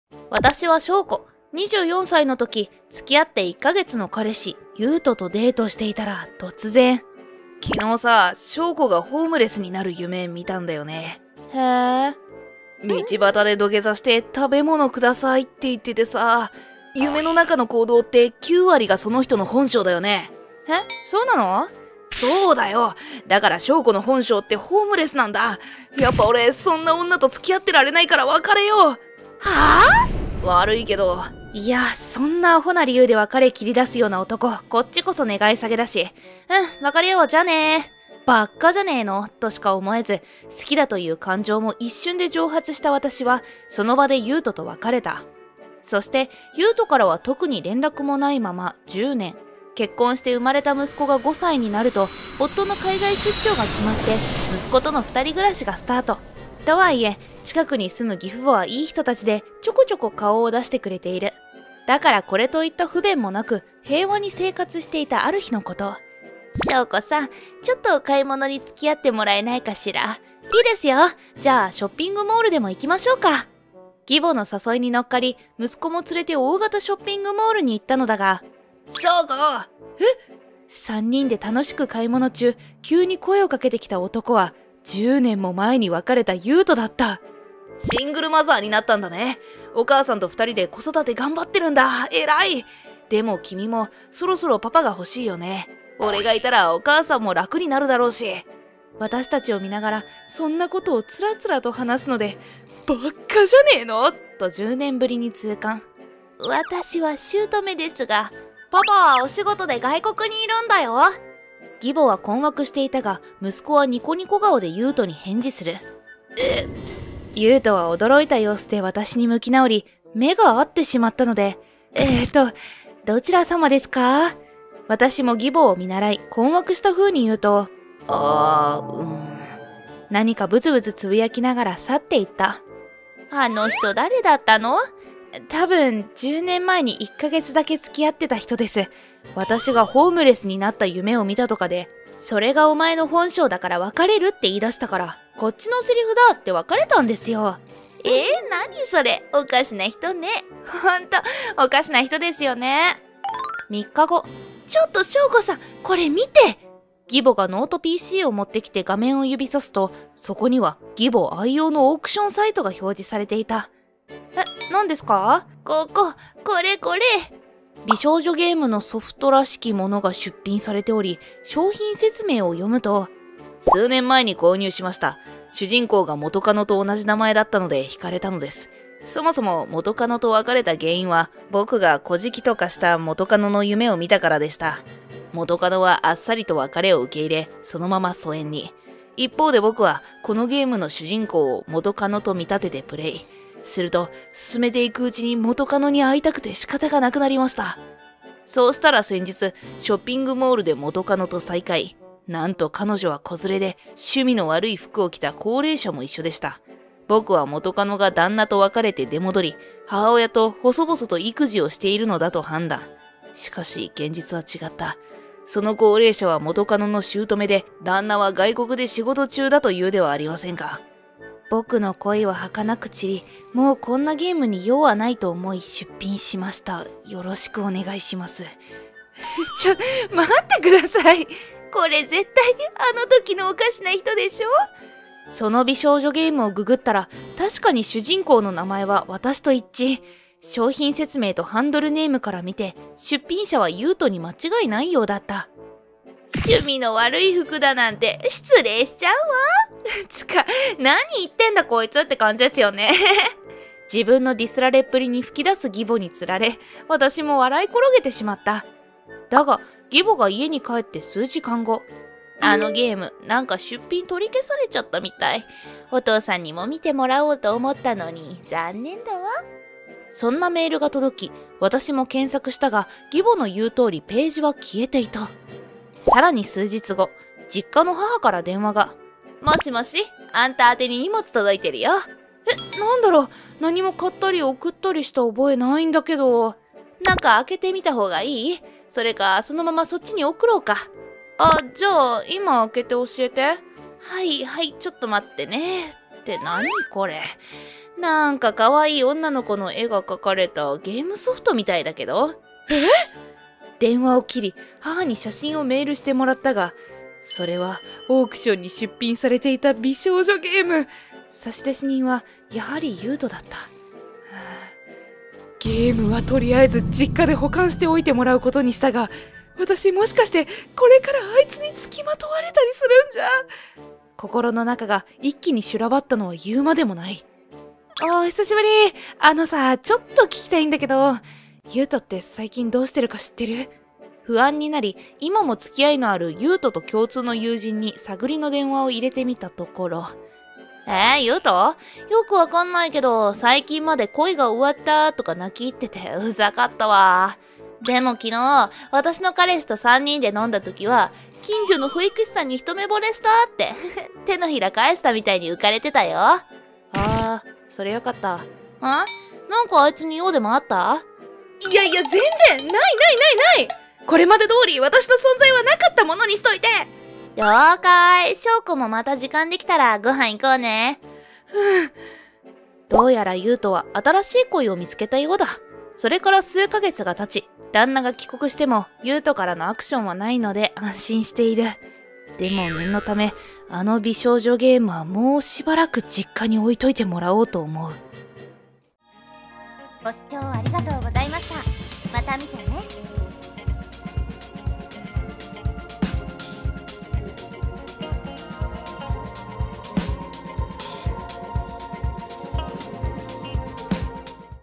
【漫画】彼氏「夢の中でホームレスになってたから別れる！」意味不明な理由で10年前に別れたアホな元カレ→結婚し姑と子供とショッピングモールに行くと元カレに遭遇し…（スカッと漫画）【マンガ動画】
〜あらすじ〜
「夢の中でお前がホームレスになってたから別れよう」
10年前に別れたアホな元カレとは意味不明な理由で別れた。
それから数年後、無事に旦那と出会い結婚。
子供にも恵まれたところで夫の海外出張が決まり、いい人な義両親のサポートもあり幸せな生活を築いていた。
そんなある日、姑と子供と三人で行ったショッピングモールであの元カレと遭遇し、何故かシングルマザー認定され…。